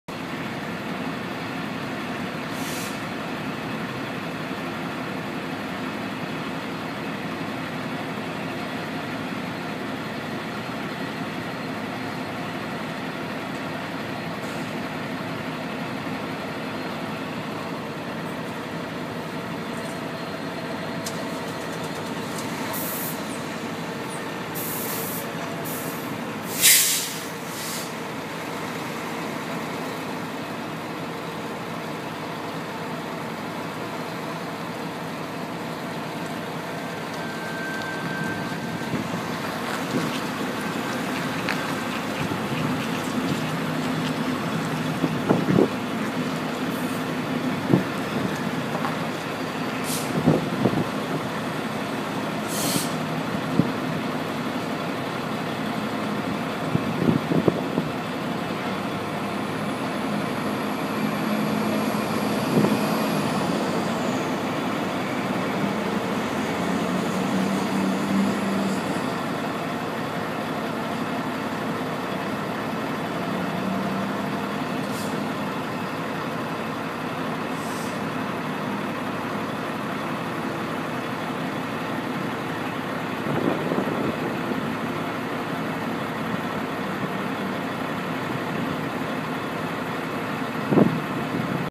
EWS 66 077 at Hull Docks 22/5/2012
EWS 66 077 at Hull Docks 22/5/2012 shunting steel carriers on Hull Steel Terminal.